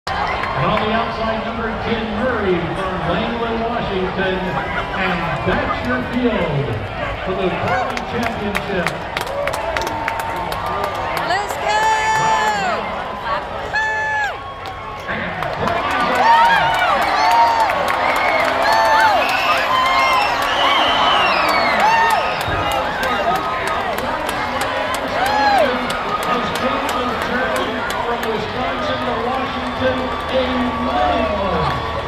Championship Heat Corgi Races Emerald Downs 2024
The Corgi Races at the Emerald Downs Racetrack in Washington State is possibly the most joyous day of my whole year. The little dogs are just so happy to run and meet other people and corgis, the announcers are just as serious as if the dogs were thoroughbred horses, everyone has a wonderful time. This year, one gray with black speckles was so excited, they made a break for it, trying to run the entire horsetrack. the owner literally had to dive and tackle the dog into the dirt. Neither were harmed, and Ahlzeimer's research recieved some major funds. Over 100 Corgis ran in this year's race, here is the Championship Heat, presented for your enjoyment. You will believe a Corgi can defy gravity.